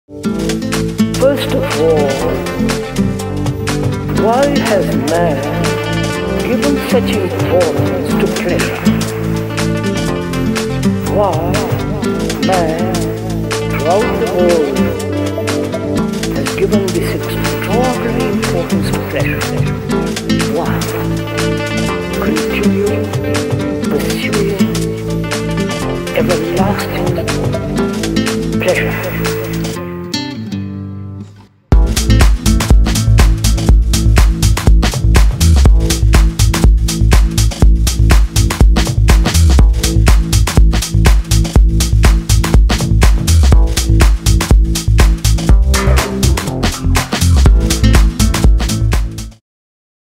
Sunscape Festival 2017: Atlantis Calling - Intro
3 days & nights of Partying by the Beach, Connections, Art & Inspiration. Over 60 Artists, Healing, Camping & so much more to be discovered.